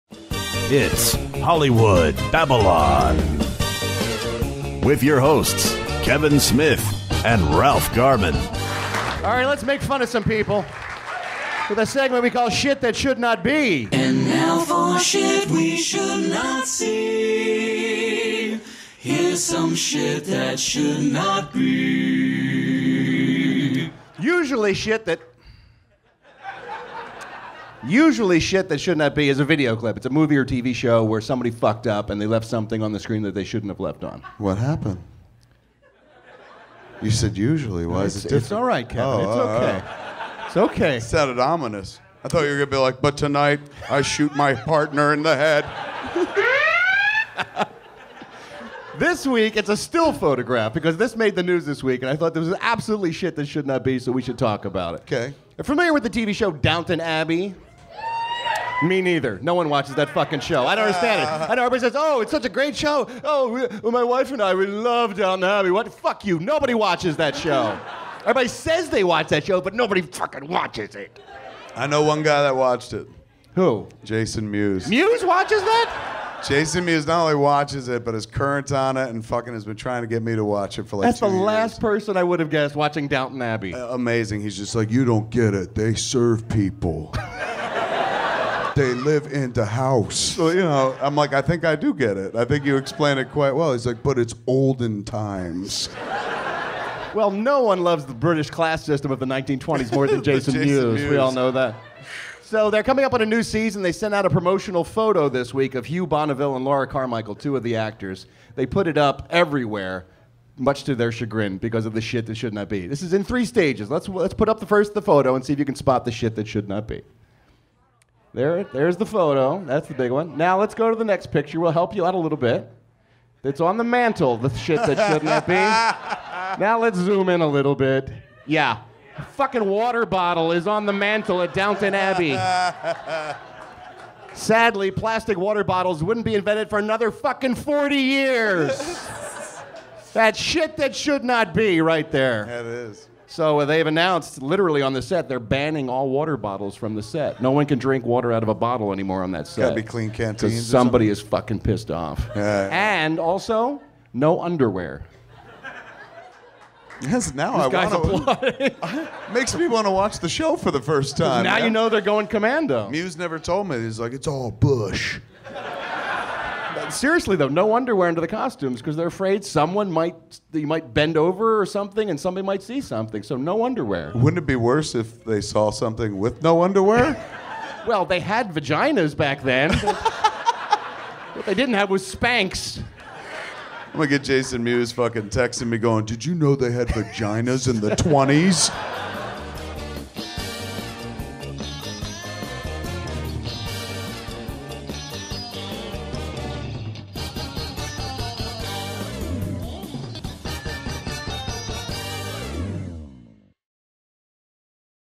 Downton Abbey Bans Water Bottles and Underwear
Nobody watches Downton Abbey...except Jason Mewes. And now for some sh*t that should not be.